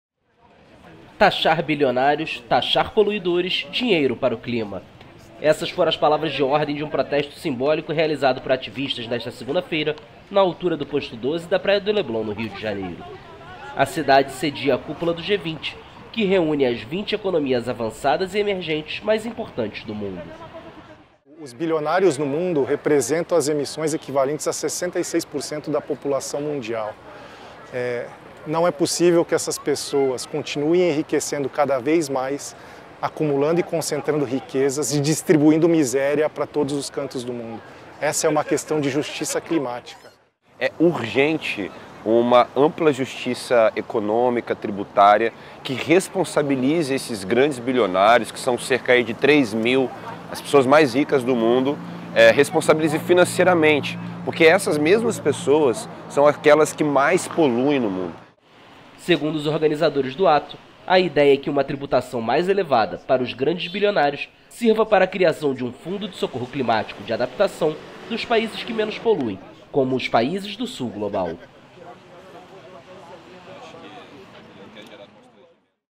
Protesto às margens do G20: 'Taxar Bilionários, Taxar Poluidores, $$$ para o Clima'
Em uma manifestação pacífica, uma ONG ambientalista montou uma grande placa de papelão em forma de moeda na orla do Rio de Janeiro com os dizeres: 'Taxar Bilionários, Taxar Poluidores, Dinheiro para o Clima', enquanto a cúpula do G20 acontece na cidade.

Imagens: AFP